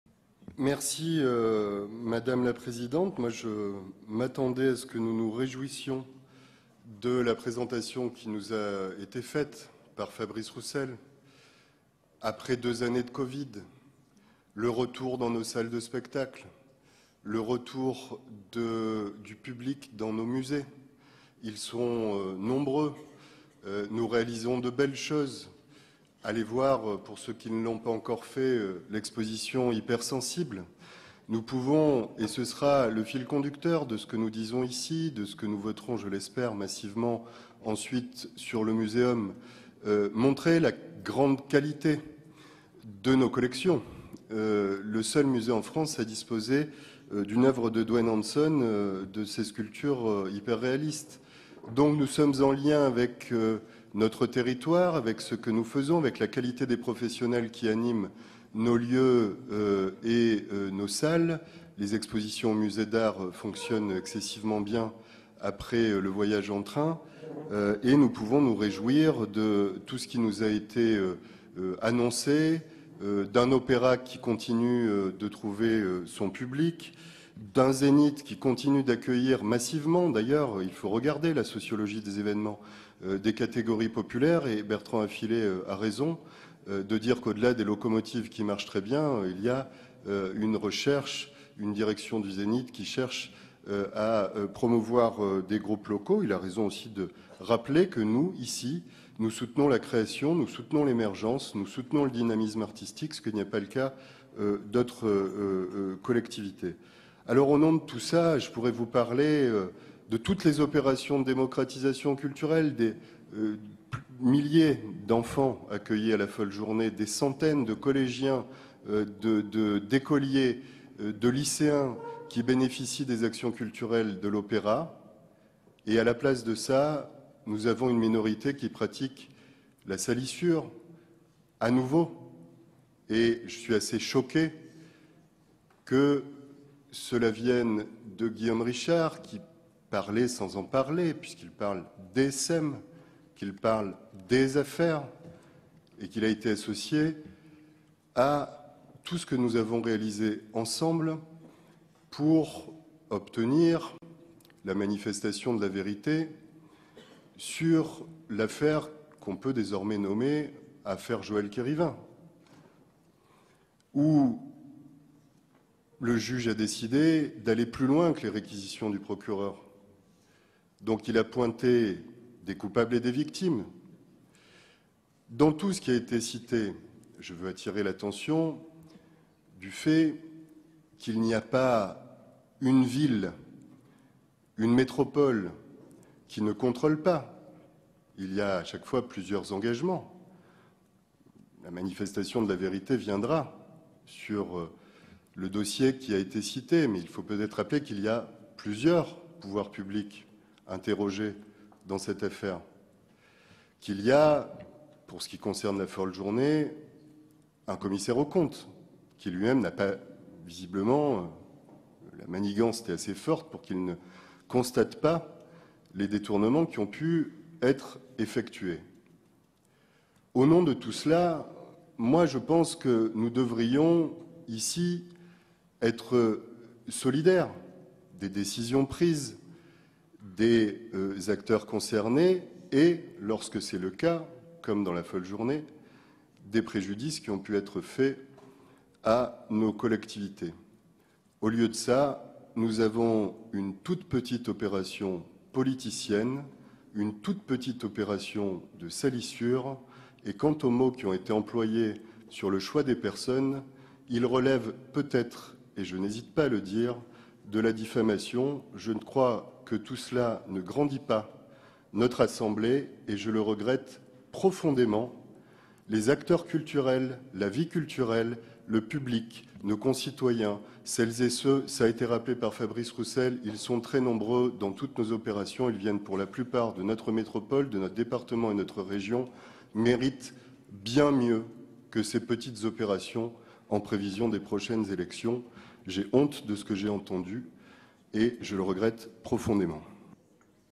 Aymeric Seassau sur les équipements culturels
Intervention d'Aymeric Seassau sur les compétences et les équipements culturels de la métropole nantaise au conseil du 23 juin 2023.